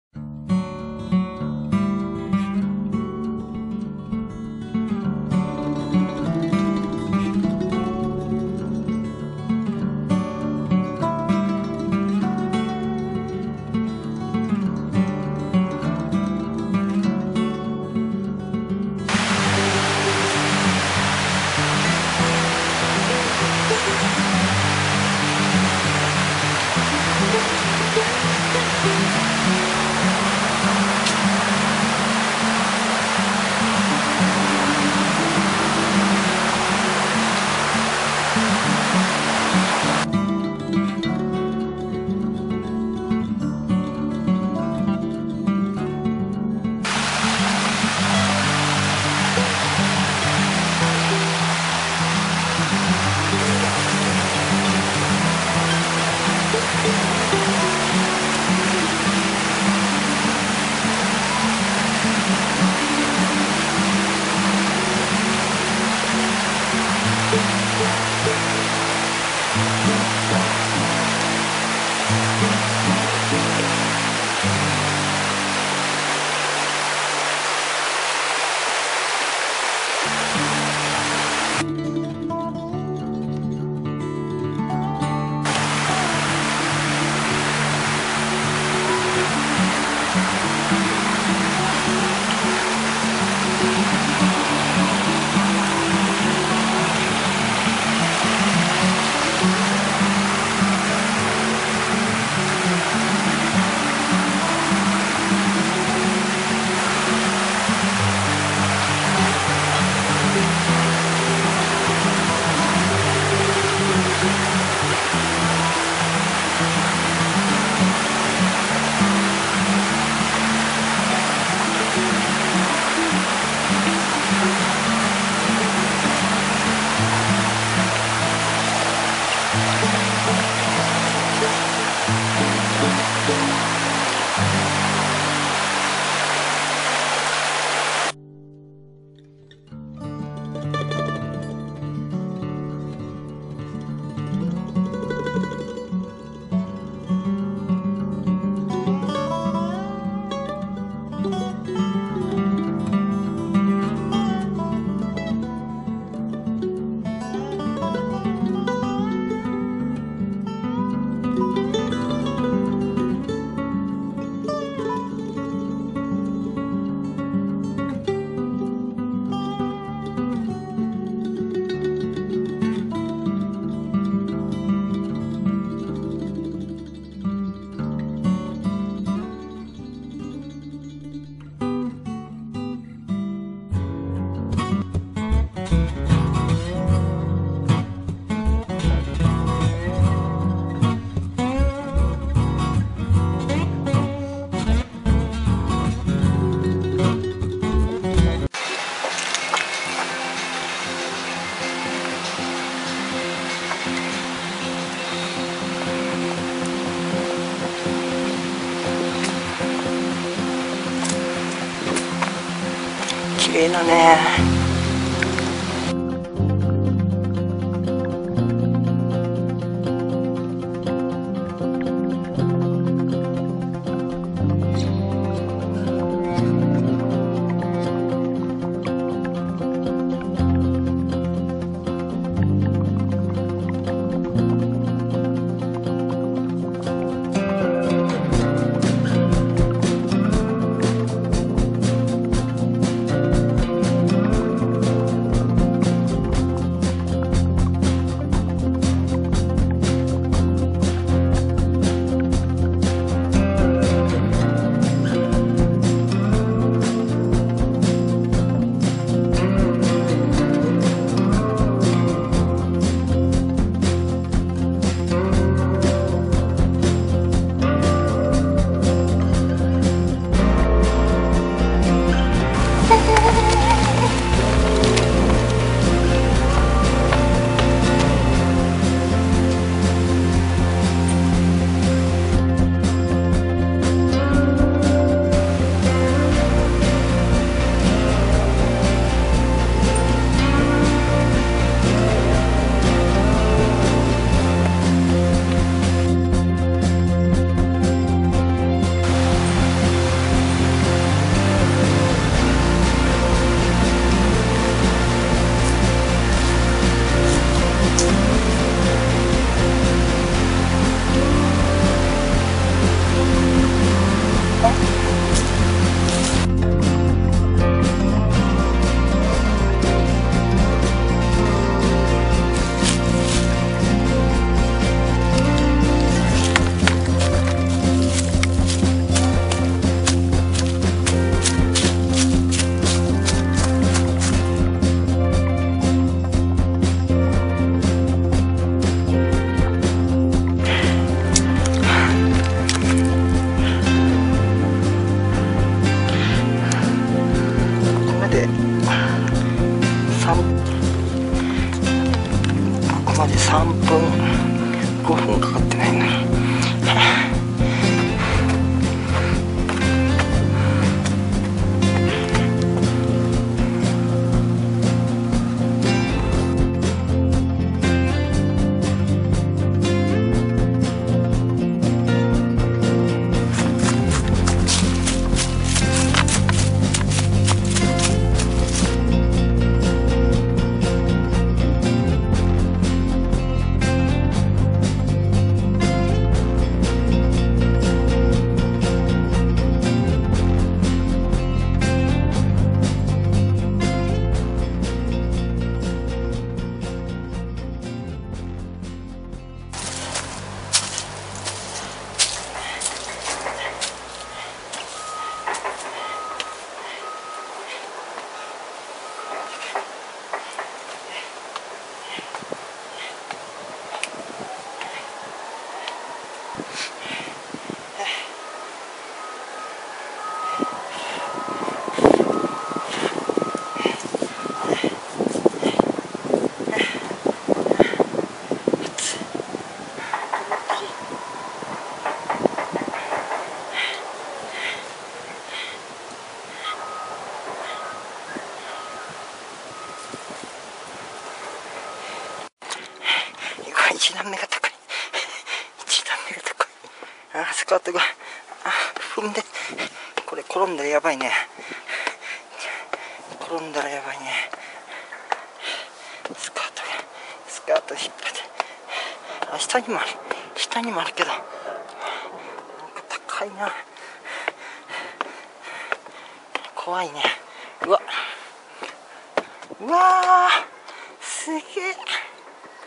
女装 de 新しく買ったスカートと、処分するスカート穿いて紅葉お出かけ
紅葉見たりプチ登山で女装中